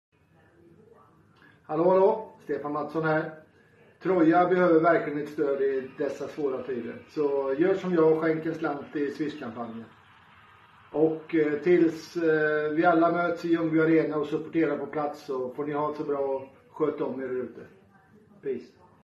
Troja: Stefan Mattsson
Red Stallions kampanj till förmån för IF Troja Ljungby. Hjälp Troja att trygga föreningen ekonomiskt genom pandemin. Vi tänker på hela föreningen, allt ifrån Tre Kronors hockeyskola, alla ungdomslag och juniorlag till A-lag för både herrar och damer.
Troja behöver vårt stöd! - Swish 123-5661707